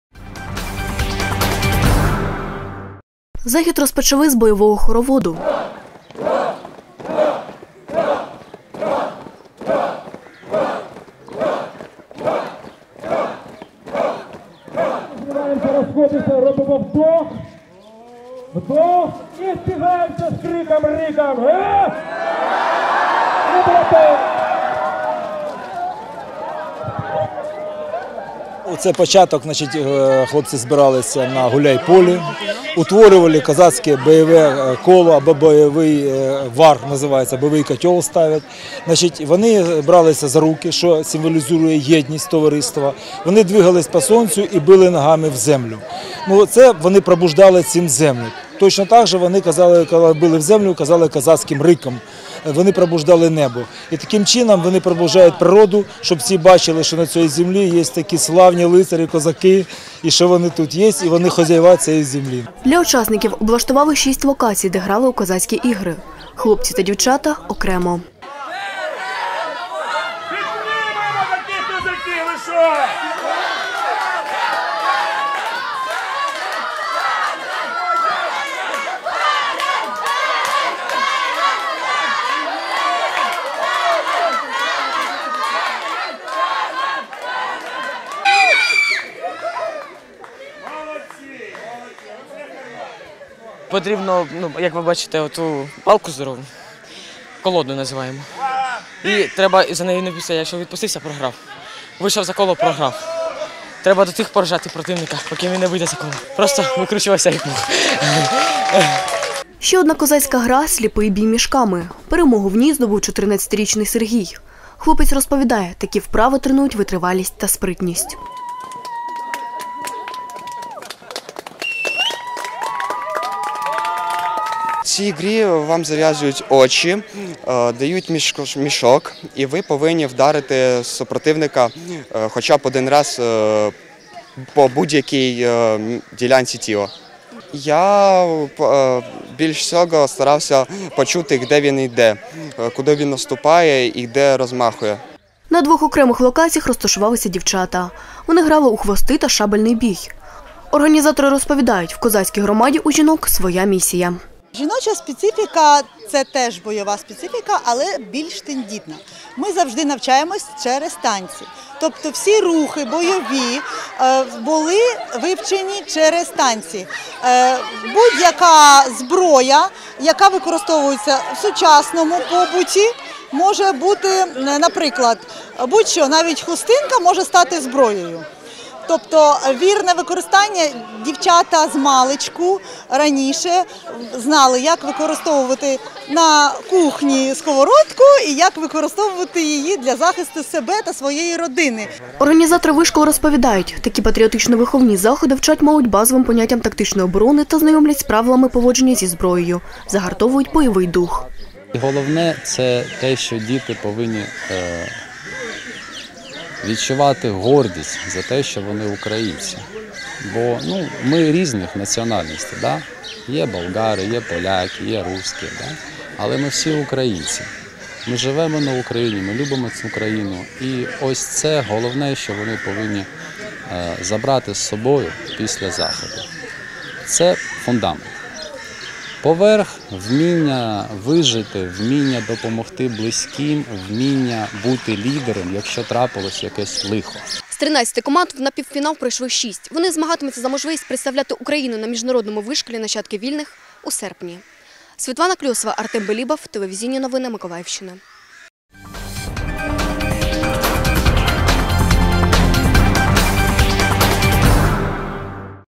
Всеукраїнський вишкіл «Нащадки вільних»
В Очакові триває Всеукраїнський вишкіл «Нащадки вільних». У рамках цього заходу миколаївська обласна школа «СПАС» влаштувала для учасників бойові ігри «Козацький грай». Підлітки з 13 областей України випробували свої сили.